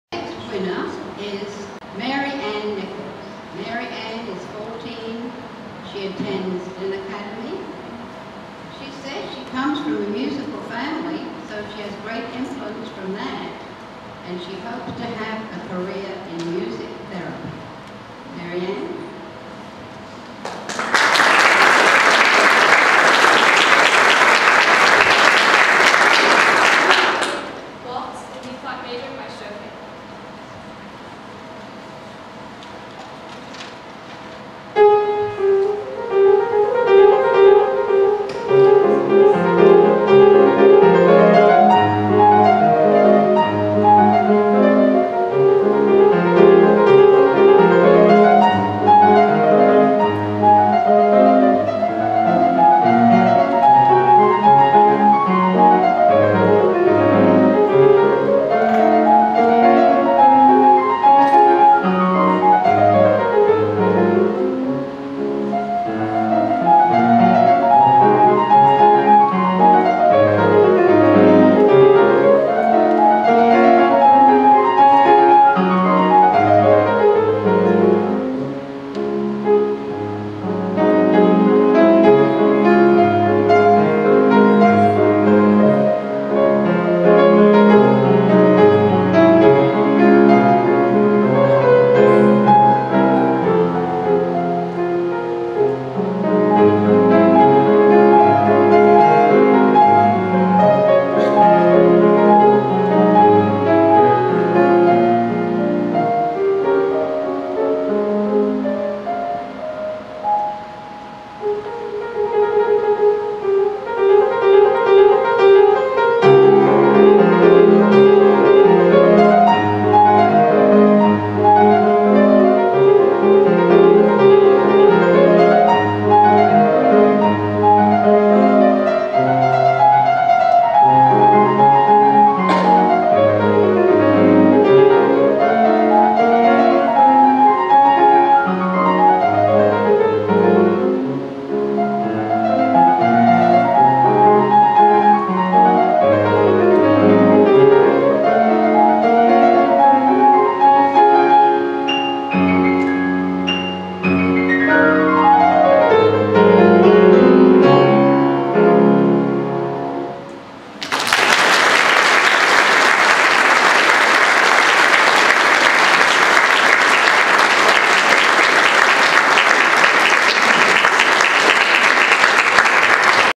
Mary Ann Nichols - 2012 Mozart Society of Glynn County Scholarship Winner
Mary Ann Nichols was a 2012 Mozart Society of Glynn County 9th Grade Scholarship Winner.  This is her performance at the Scholarship Winner's Concert on Sunday, April 15, 2012 at Christ Church Parish House on St. Simons Island, GA.